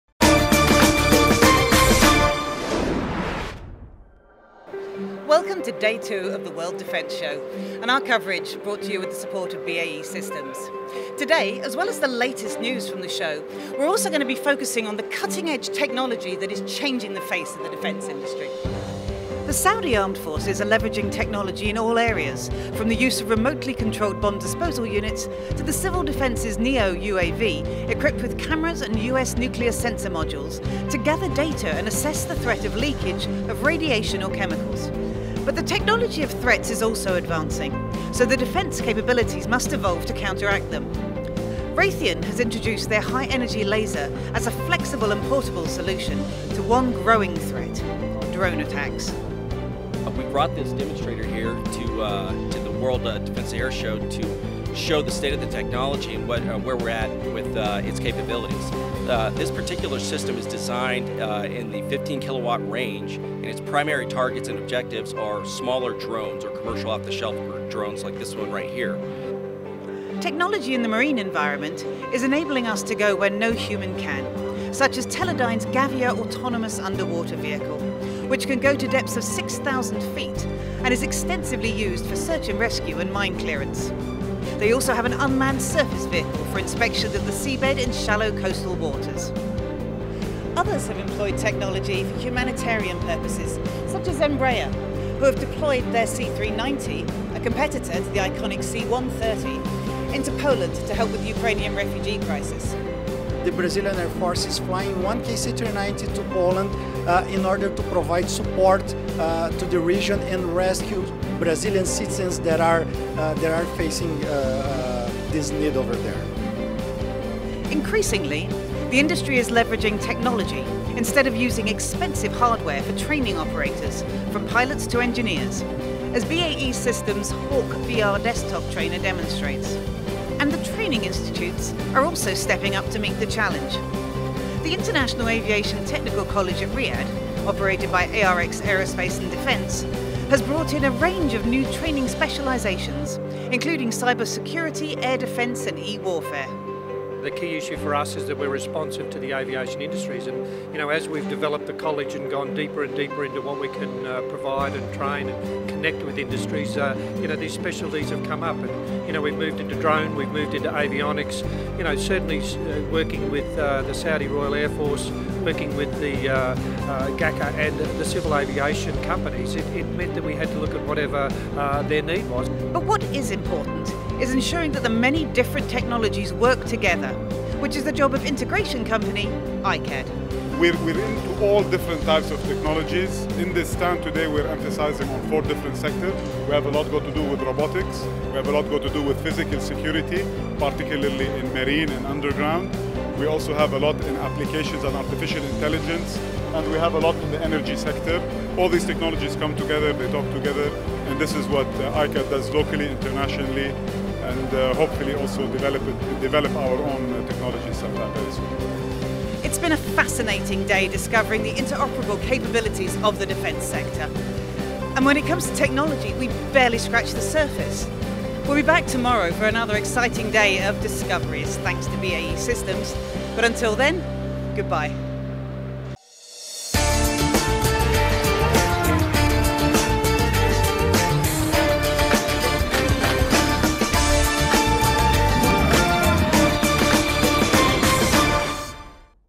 TATV event round up - World Defense Show 2022 Day 2
On day 2 of the World Defense Show, our TATV team explores some of the technology that is driving the growing interoperable capabilities of the defence industry.

We also look at how training is leveraging technology to meet the need for a skilled workforce.